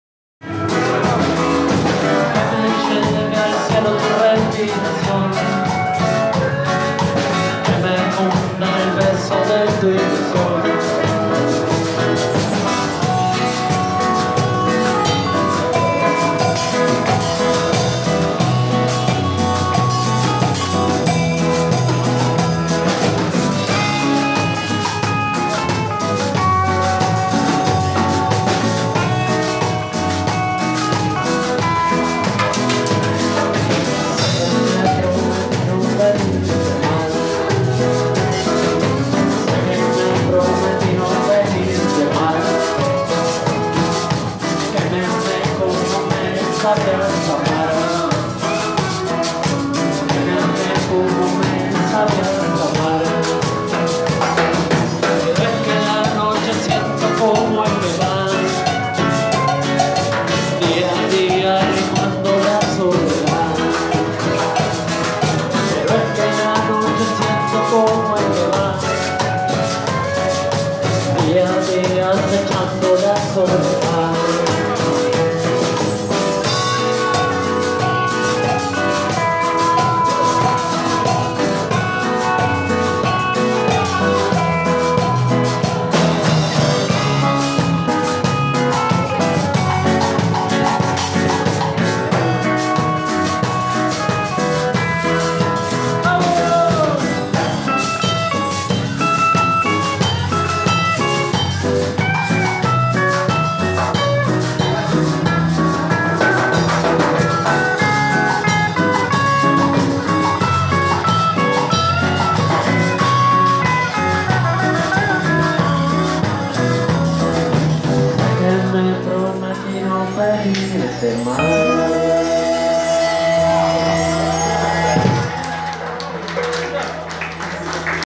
El Fondo del Tanque en La Chancha
La banda de cumbia-regaae de Traslasierra haciendo de las suyas en La Chancha Social Club!! Dia de la Primavera!!